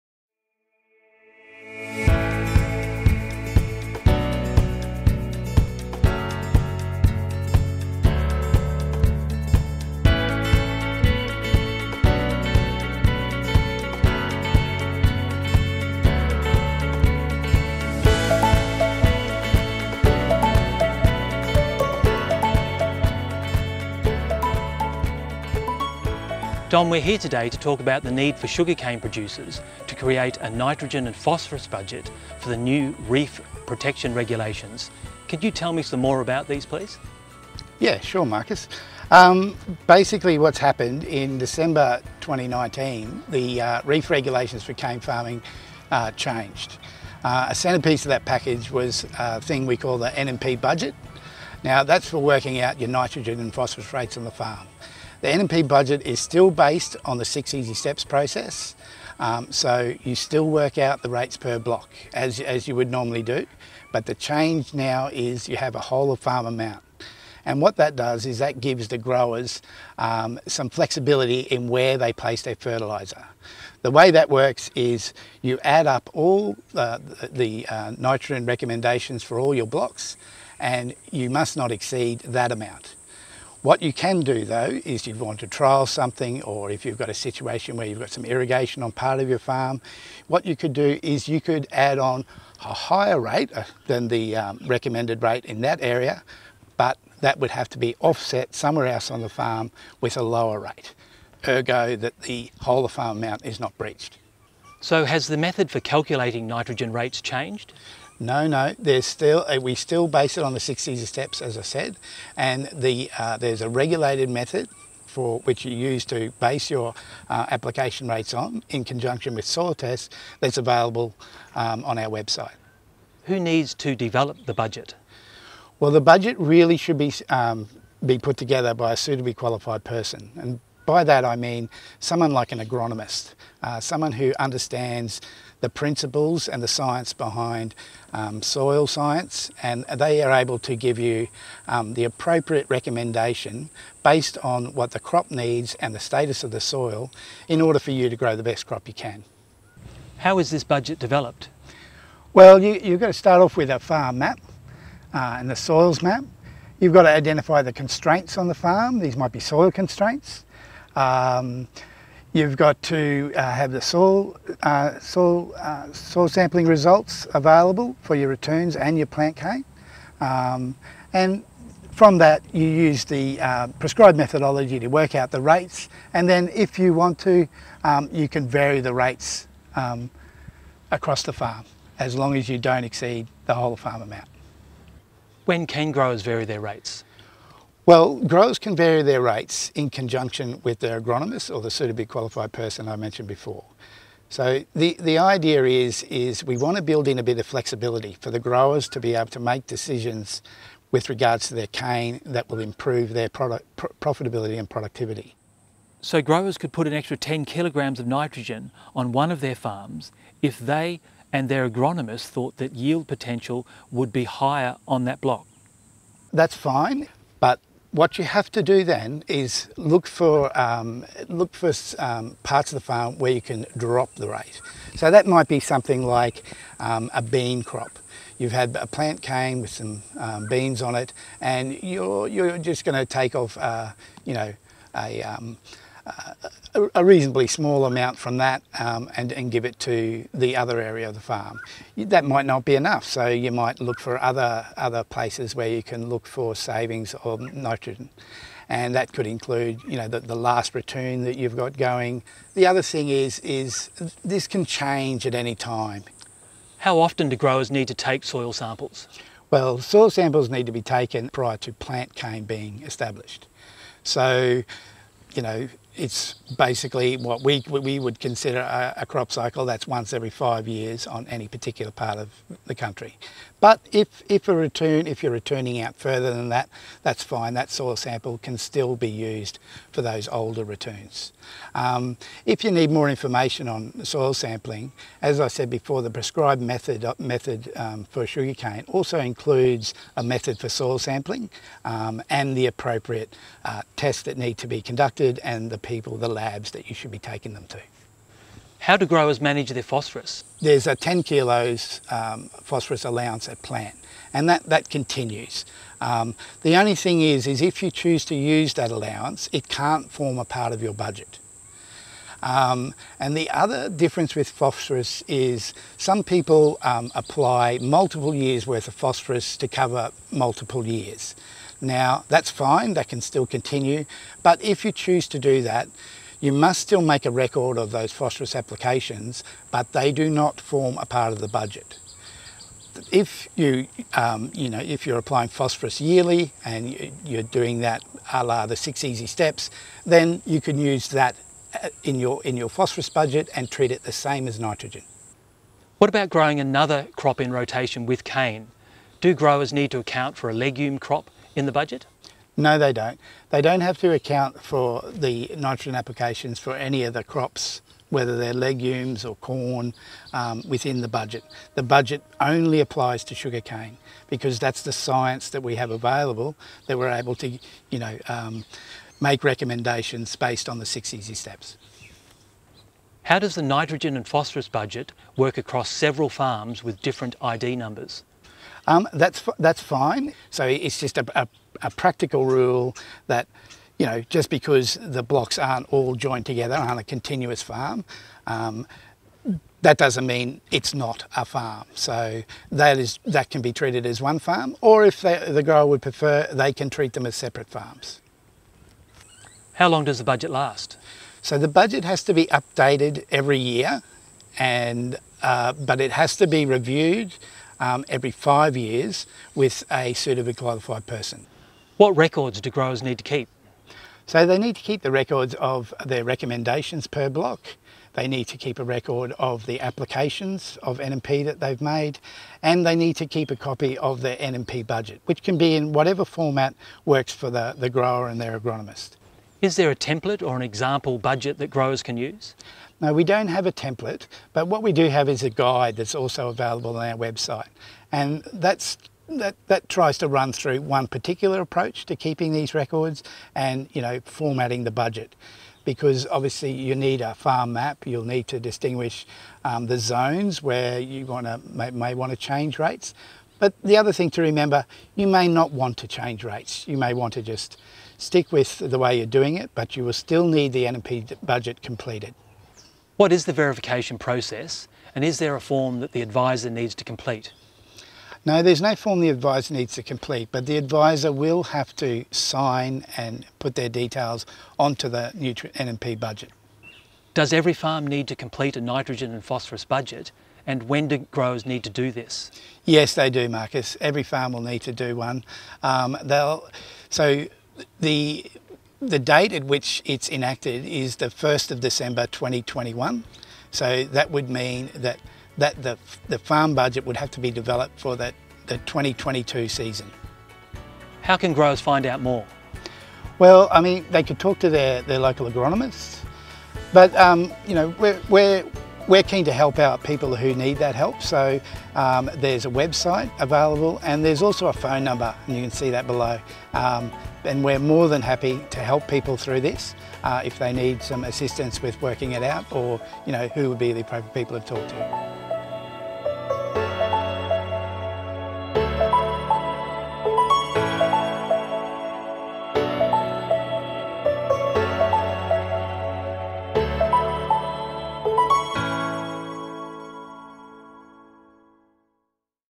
Sugarcane nitrogen and phosphorus budget
This video outlines the process to develop a sugarcane nitrogen and phosphorus budget as required under the Reef protection regulations including answering some commonly asked questions.